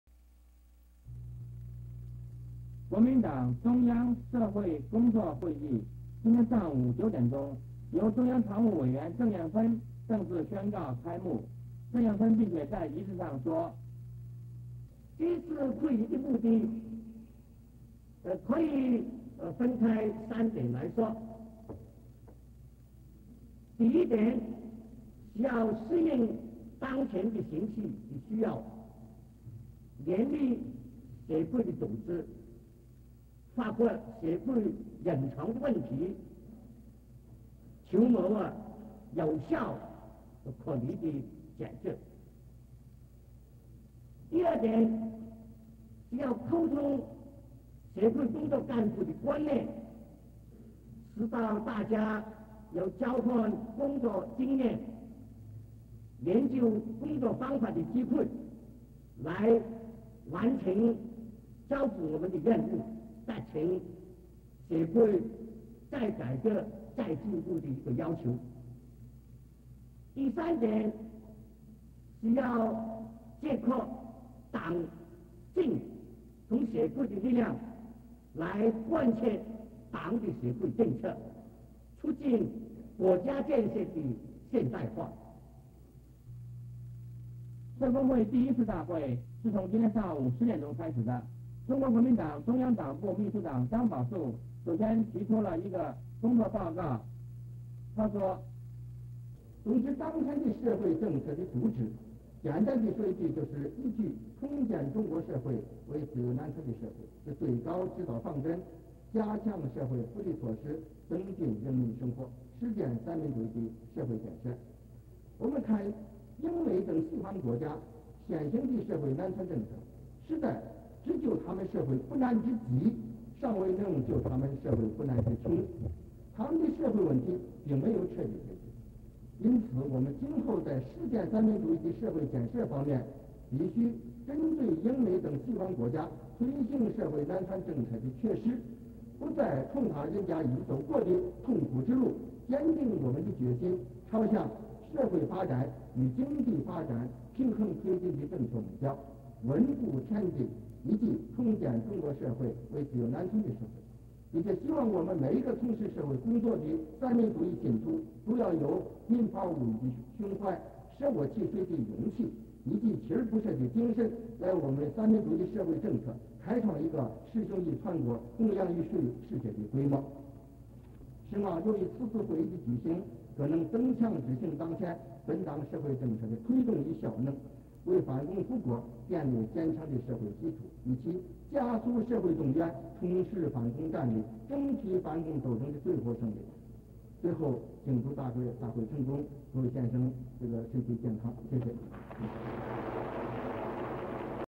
國民黨中央社會工作會議
(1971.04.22)國民黨中央社會工作會議由中央常務委員鄭世芬正式宣佈會議開始，並於會議中以八大指示作為未來中央社會工作發展的目標，黨中央秘書長張寶樹於會議中提出報告，要求黨的幹部要能在工作上求新求變方能滿足社會的需求及民眾的期待.(有聲)